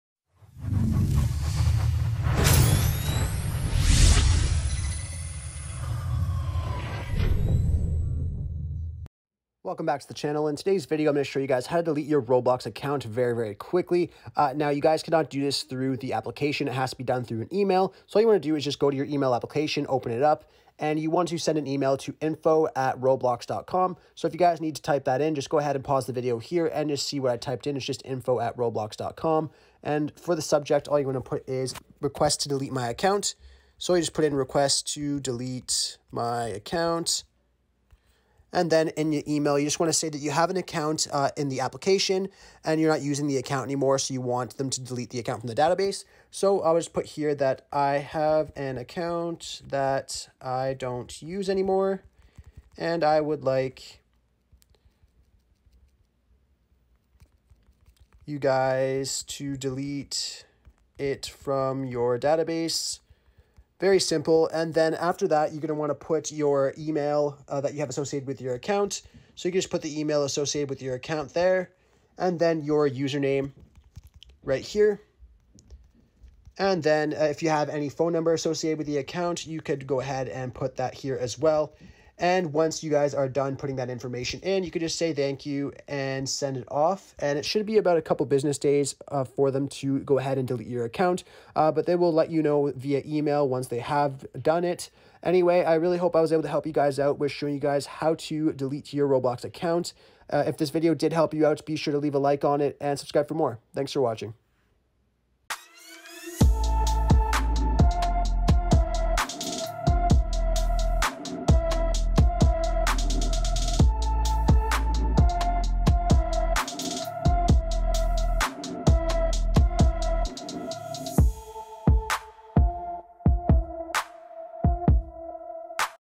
How To Delete Roblox Account on Mobile 🎮| Delete Roblox Account iPhone
In today's video we show you How To Delete Roblox Account on Mobile.  It's super easy to fix and it takes less then 2 minutes.

Click Here For More Roblox Tutorials/Tricks!